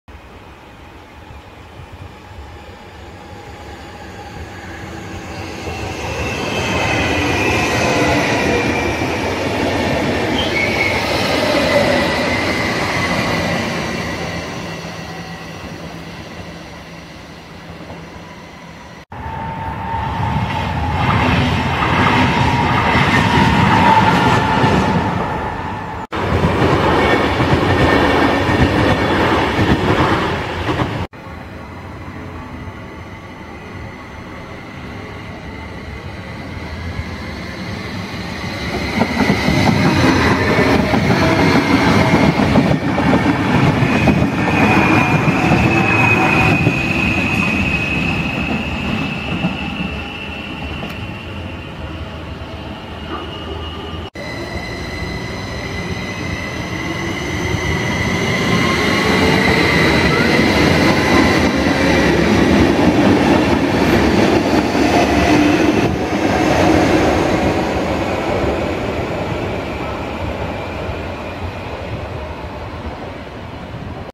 Züge in Uster - Treinen in Uster, Zwitserland
Wat treinen op en rond het station van Uster, Zwitserland.

Some trains at and near the train station of Uster, Switzerland.